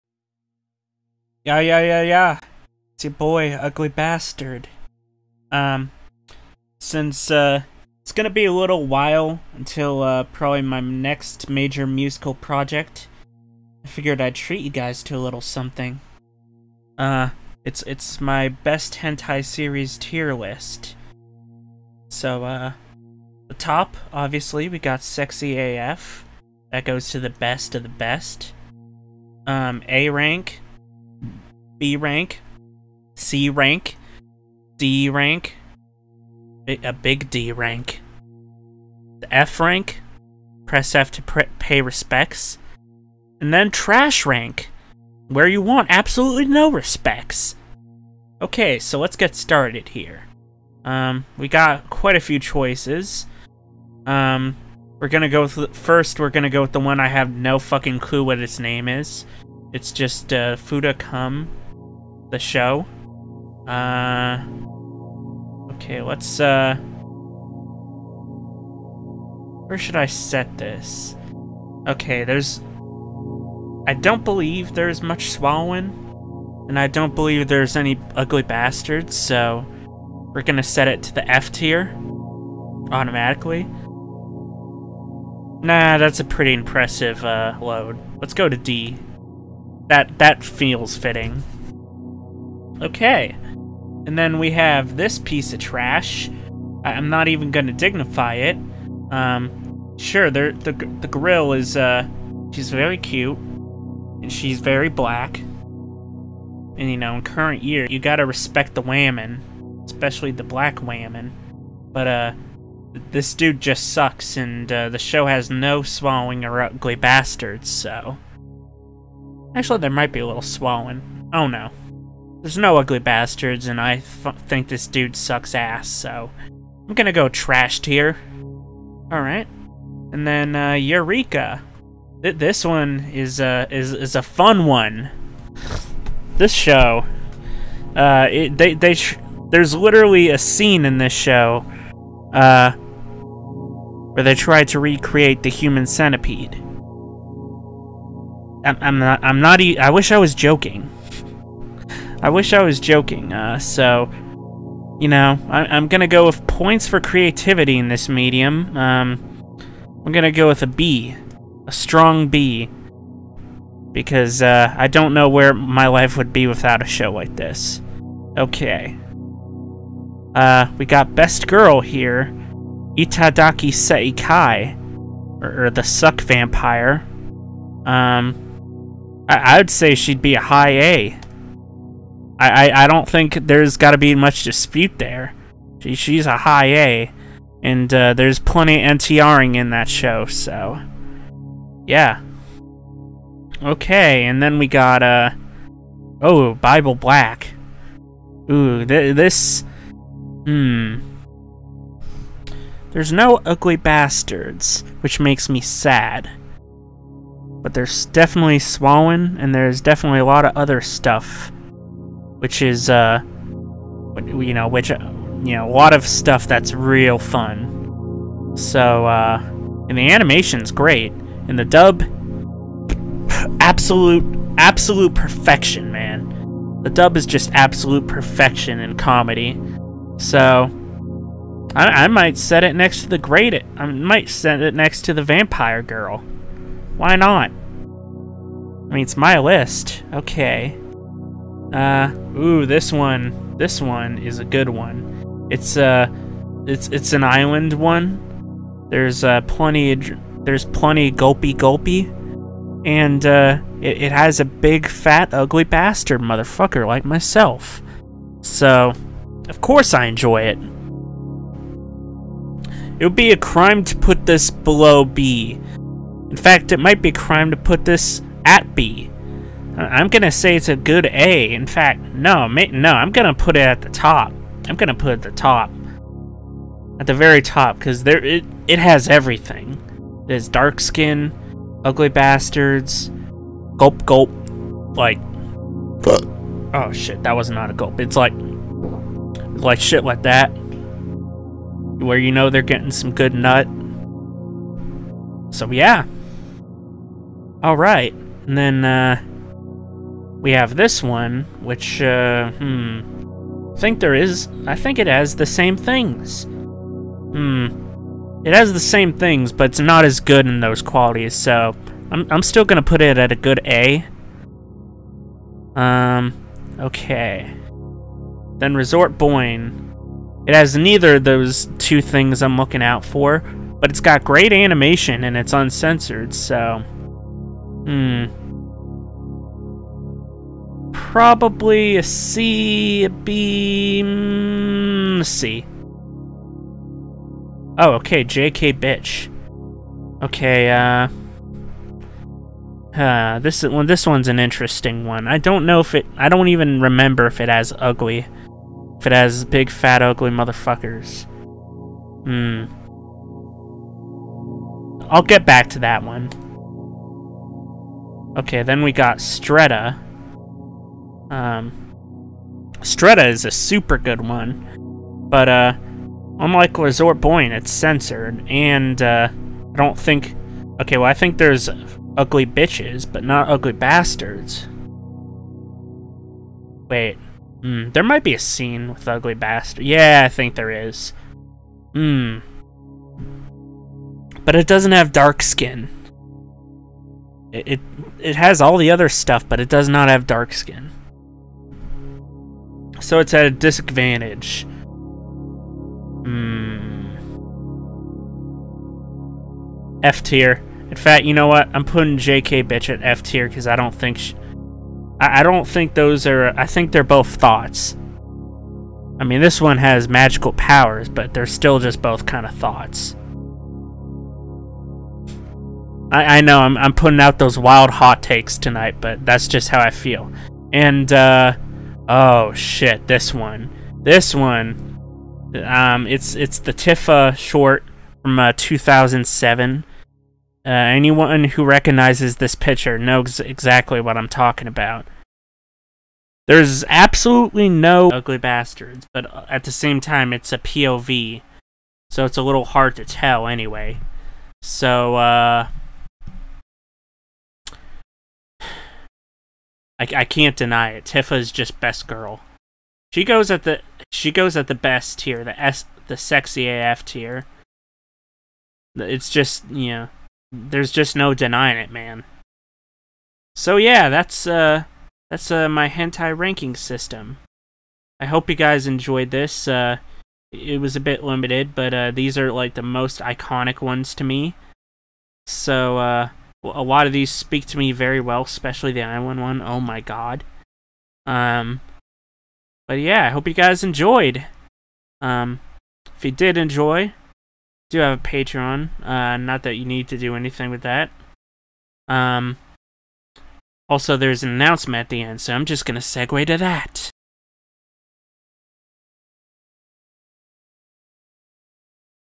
Spicy Anime Tier List (Top 11 Best H Anime Series)
Here's my tier list, aka the top 11 best anime for f@pping, also please watch until the end because I have an announcement! Anyway, I decided that i'd have a little fun with this video by making it a waifu / series tier list of the best anime of the lewd variety since we're all under corona-chan's thumb. Especially since my next big musical project which is being edited by H3ntie69 will take a couple weeks, so I figured i'd do this instead.